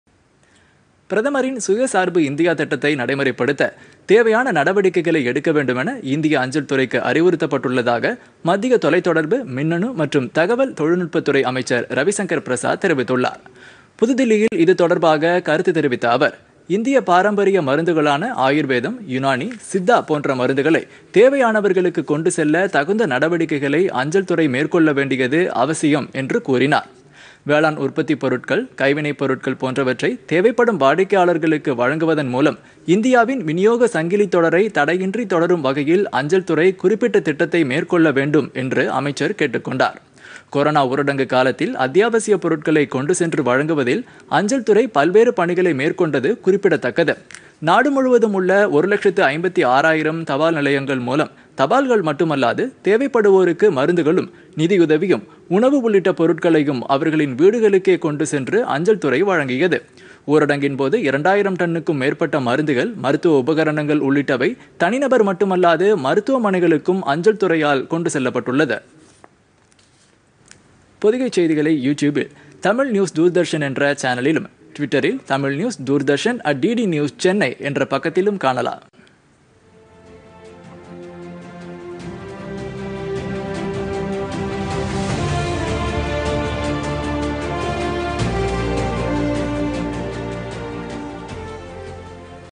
அஞ்சல் துறையின் பங்கு அளவிடற்கரியதுPodhigaiTamilNews
கொரோனா ஊரடங்கு காலத்தில் மருந்துகள், அத்தியாவசியப் பொருட்களை கொண்டு சென்றதில் அஞ்சல் துறையின் பங்கு அளவிடற்கரியது - மத்திய அமைச்சர் ரவிசங்கர் பிரசாத்.